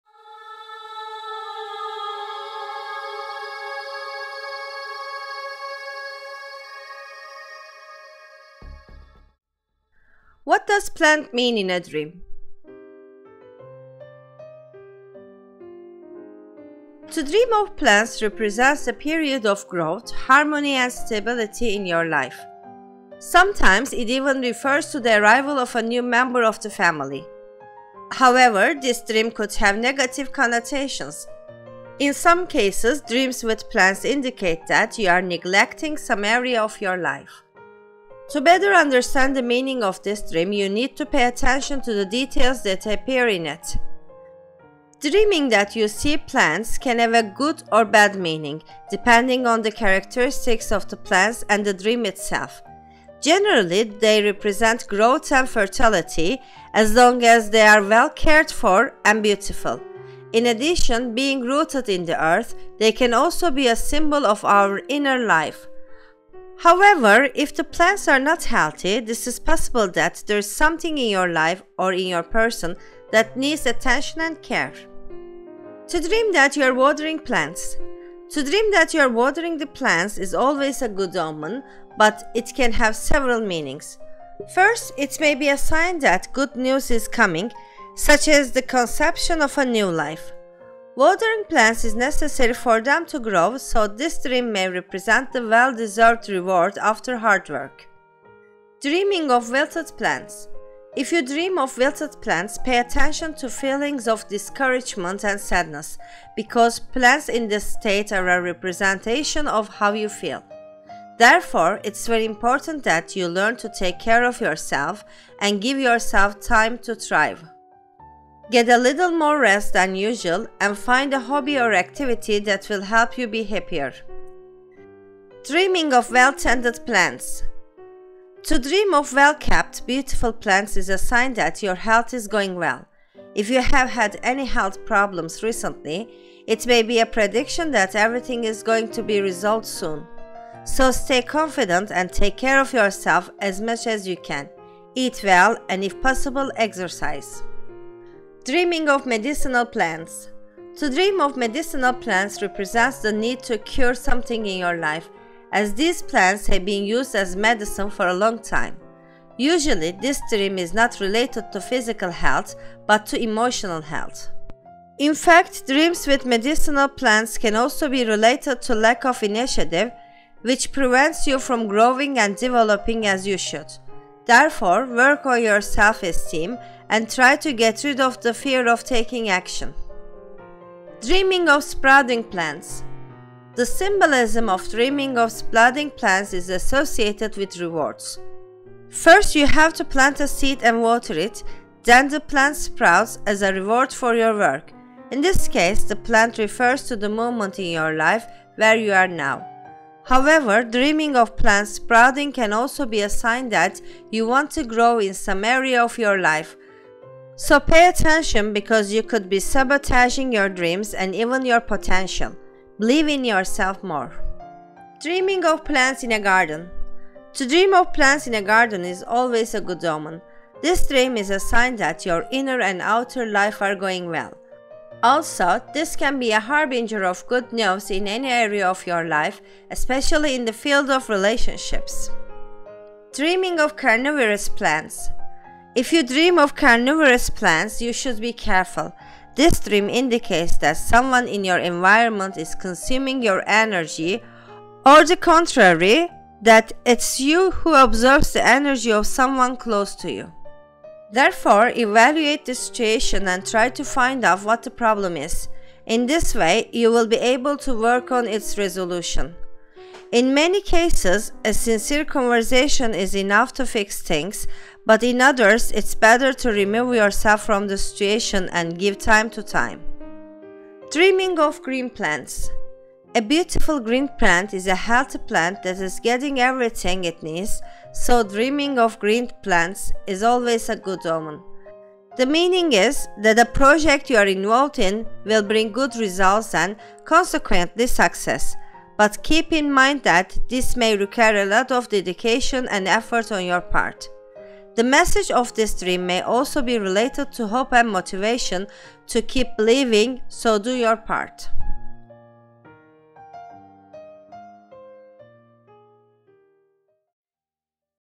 What Does Plant Mean in a Dream? Meaning of Dreams about Plants
What does it mean to dream about plants? Interpretation of dreams about plants, watering plants, green plants, plants in the garden and more.